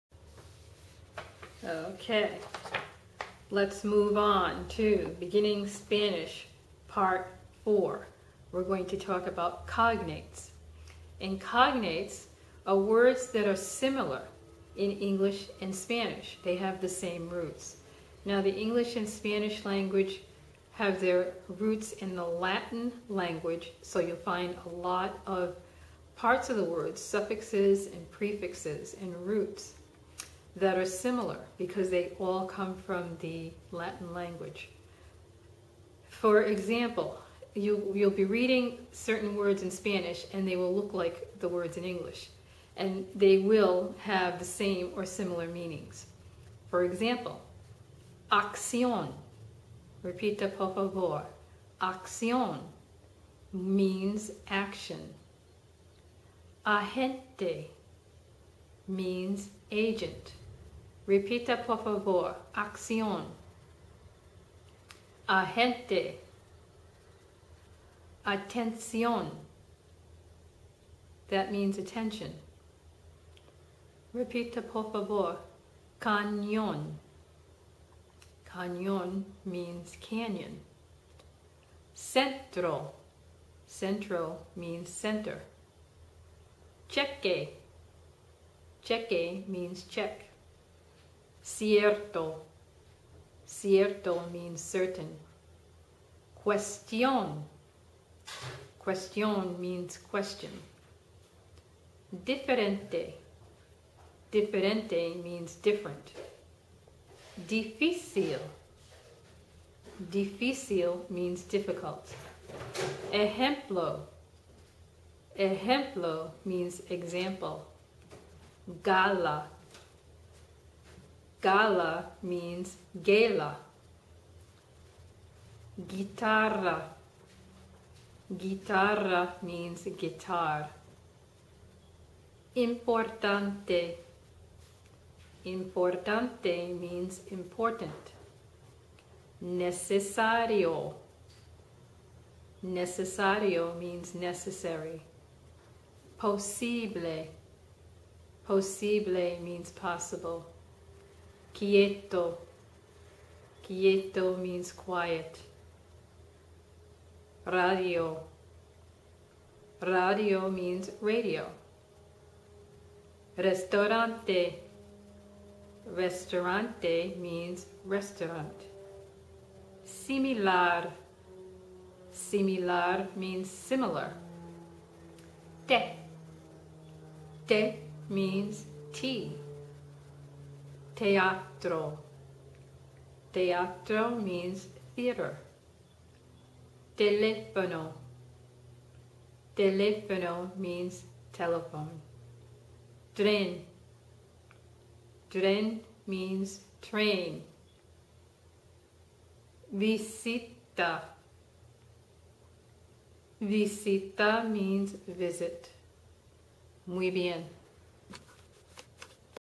Beginning Spanish - Cognates
Derived from the Latin language, cognates are words which sound and look the same in English and in Spanish.  Here you will have anopportunity to pronounce some of the words.